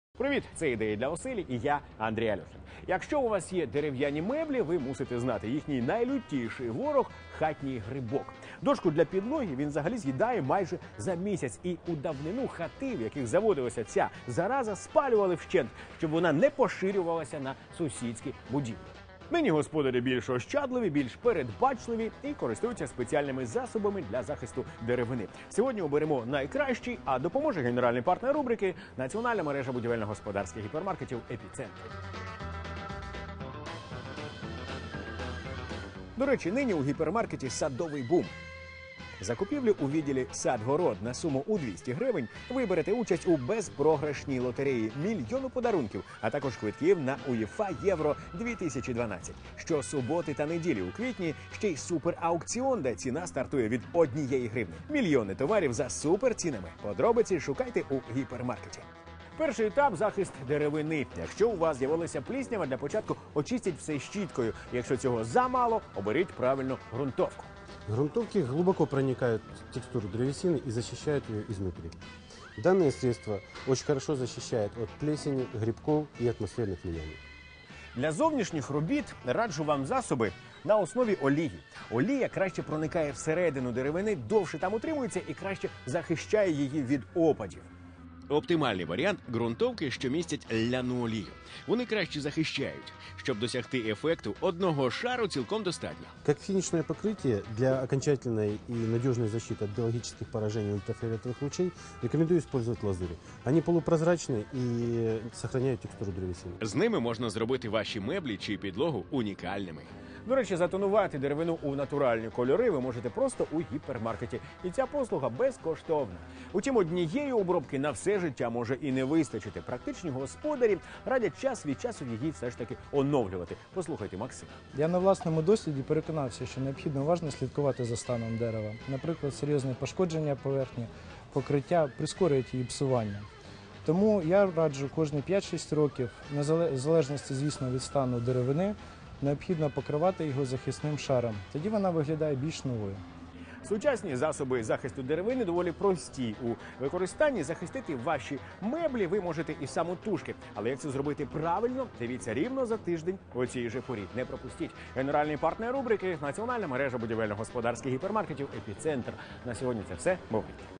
Современные методы защиты древесины
ubr.ua - украинский бизнес ресурс : &#65279;Практические советы, как защитить древесину современными методами. .